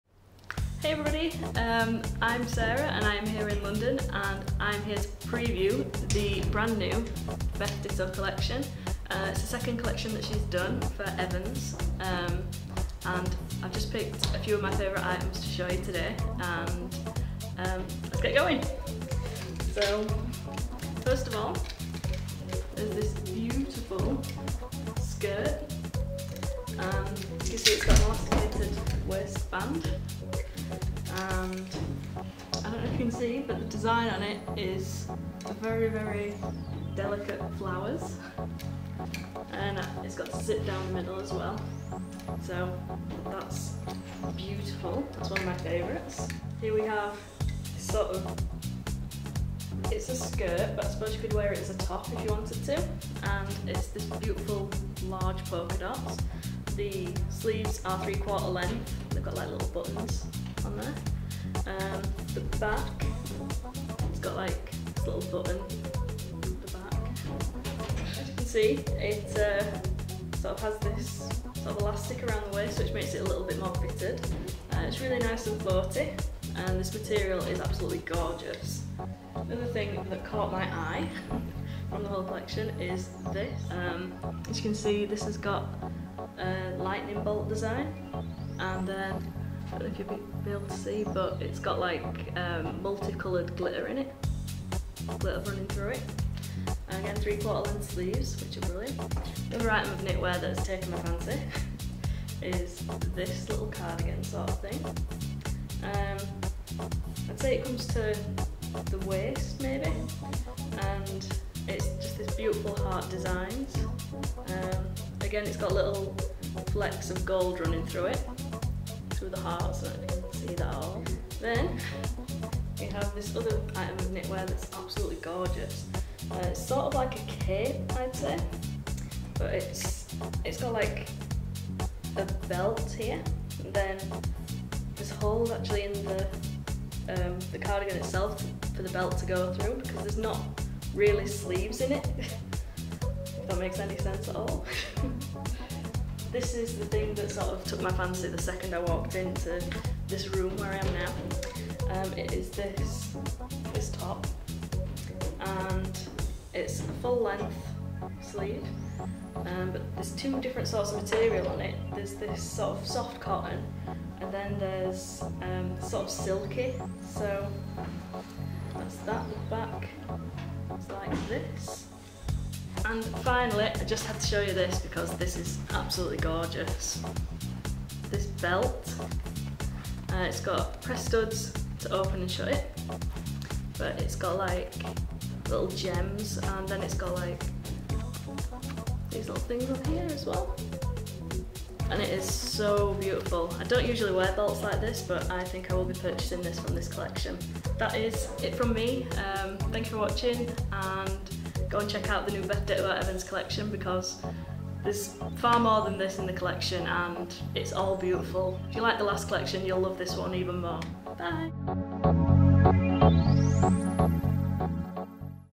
Preview The New Beth Ditto Collection | Evans
Sarah Cutts from Found on Film talks you through Beth Ditto's Autumn 2010 Collection.